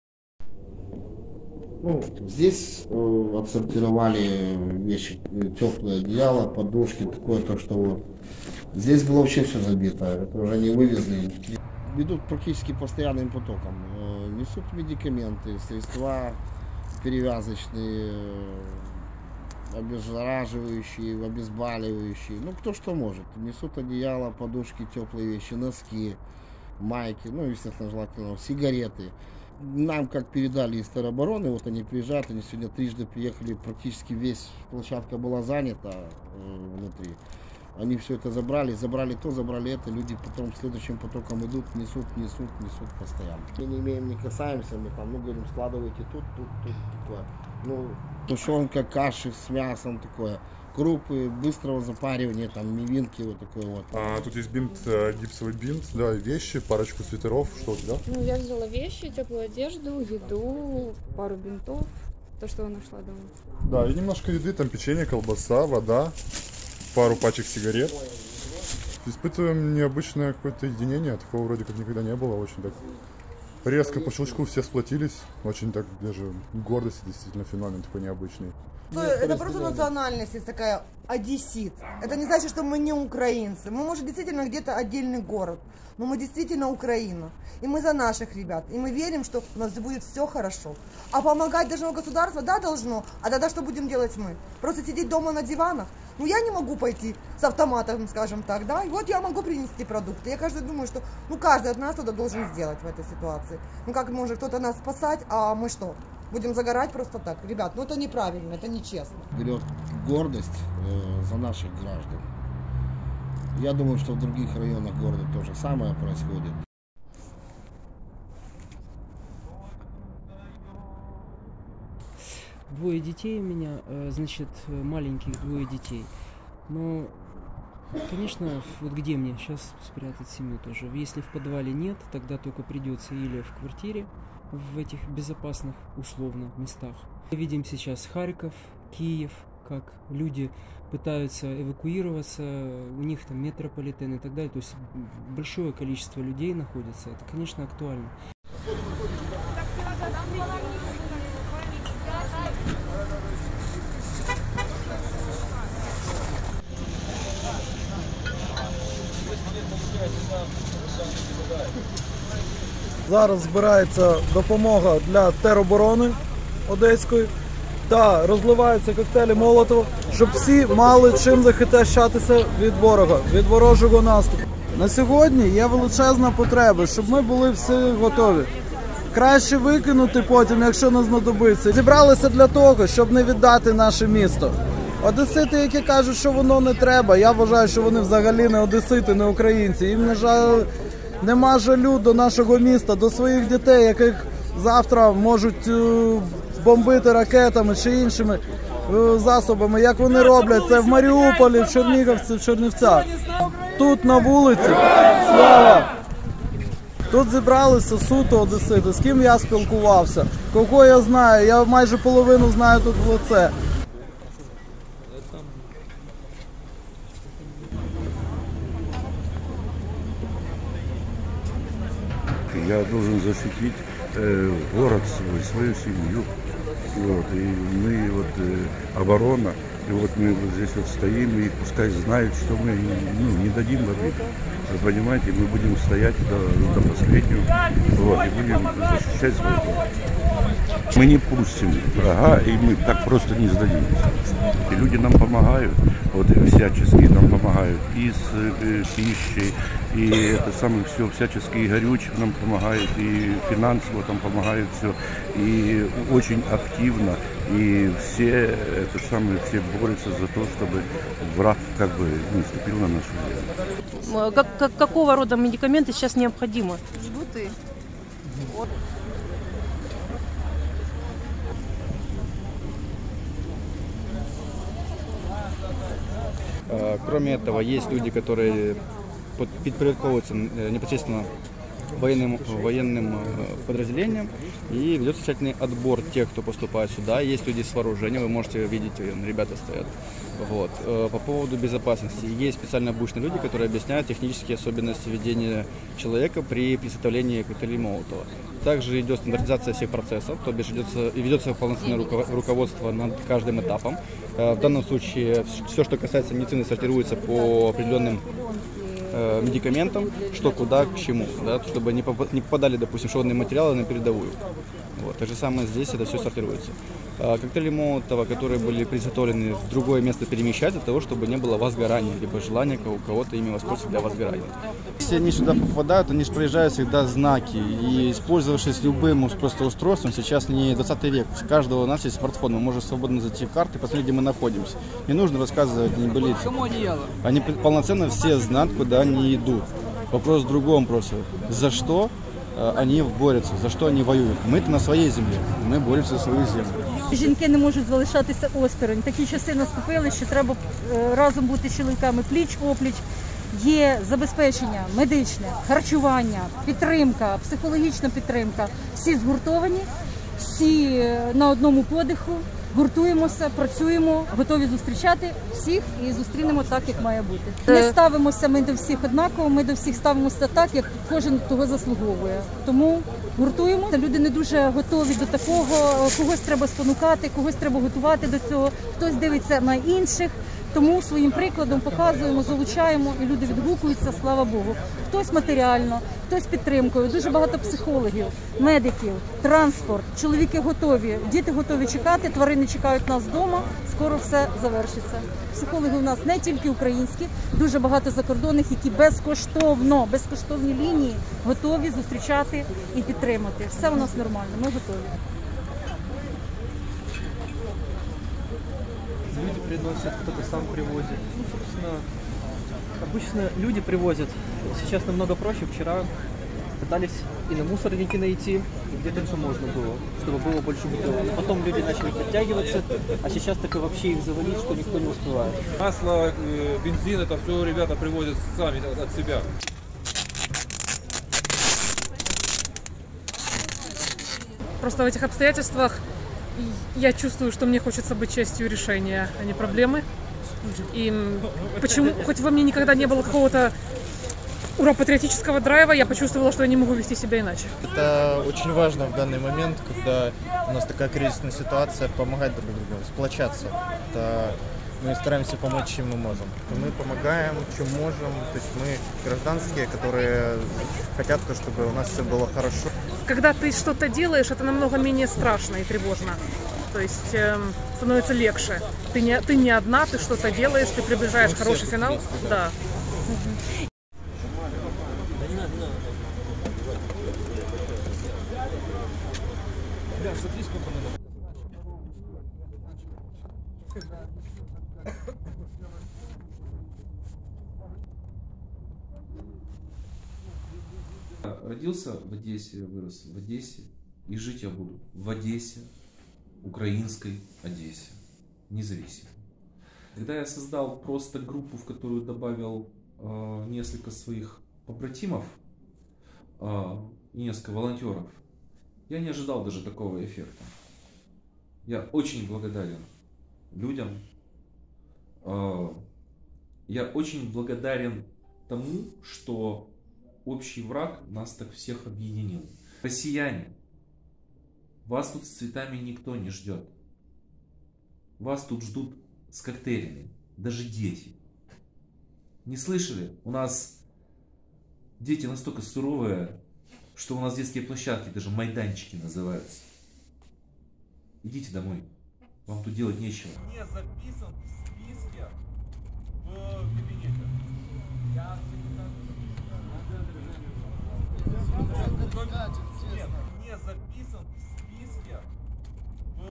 Одеса готова ефективно протидіяти ворогу
Розмірковуючи на тему сьогодення і майбутнього української мови в головному місті Півдня України, я лише висловив власні думки стосовно тих чи інших соціокультурних процесів, що відбуваються в місті, де мені випало народитися і прожити все своє дотеперішнє життя.